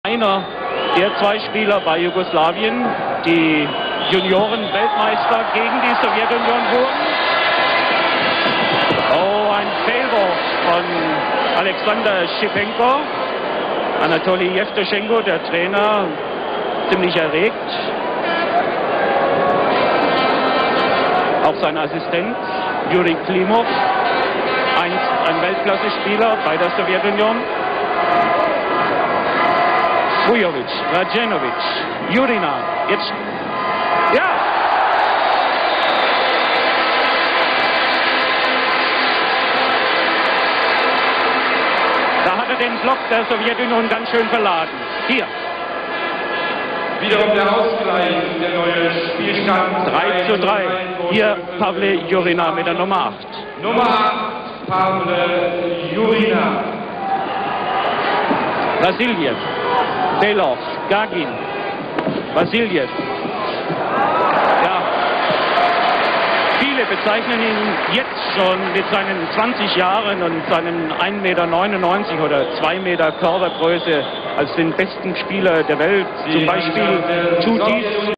Pavel Jurina Olimpiadi
Un gol di Pavel Jurina alle Olimpiadi di molti anni fa...